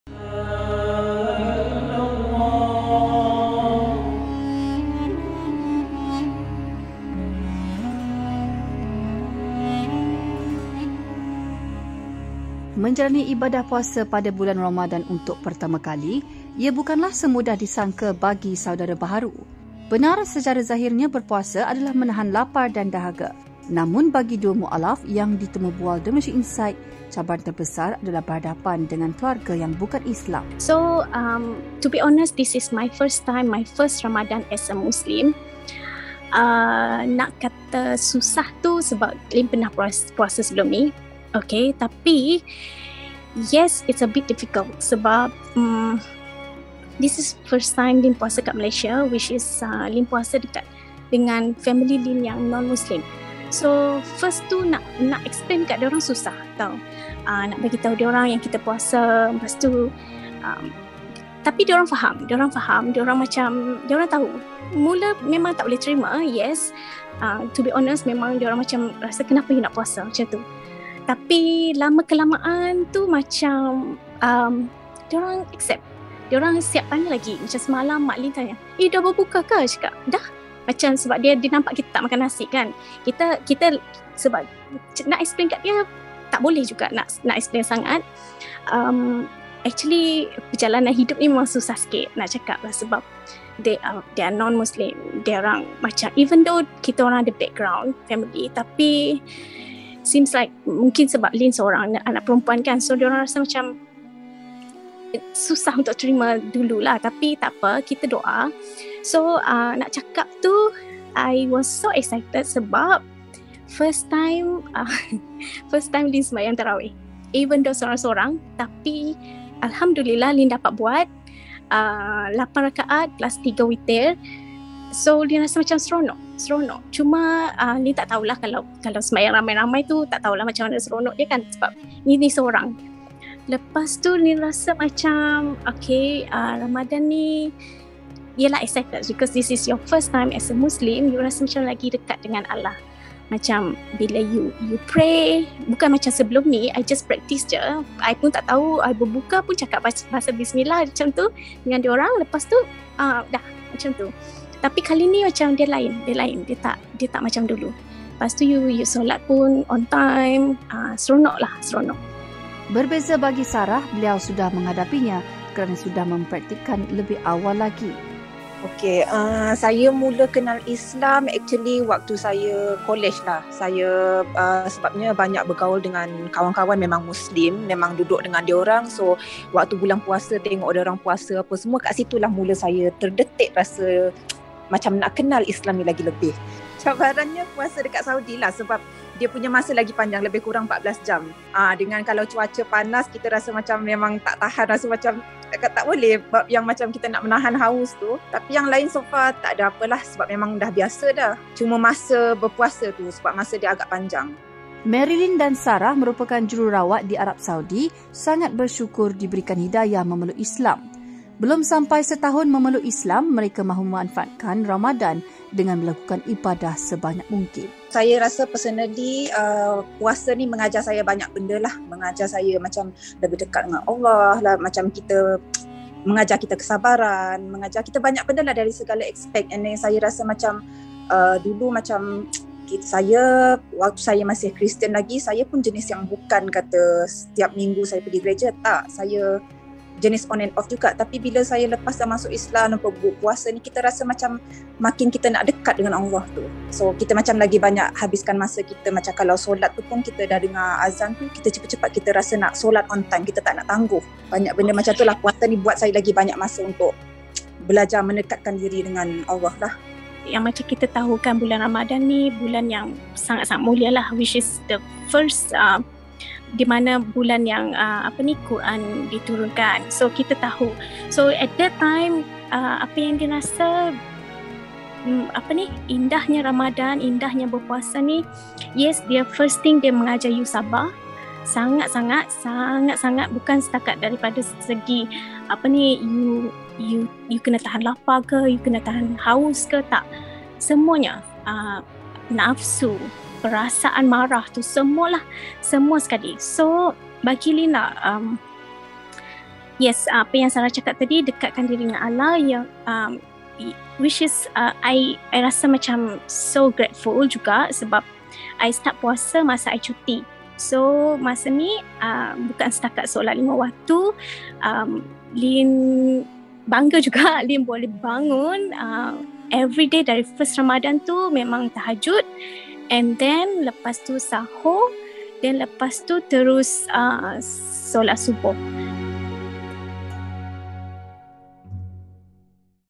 Puasa bagi mualaf, cabaran menghadapi keluarga non muslim
Menjalani ibadah puasa pada bulan Ramadhan untuk pertama kali, ia bukanlah semudah disangka bagi saudara baharu.

Benar, secara zahirnya berpuasa adalah menahan lapar dan dahaga.

Namun bagi dua mualaf yang ditemubual The Malaysian Insight, cabaran terbesar adalah berhadapan dengan keluarga yang bukan Islam.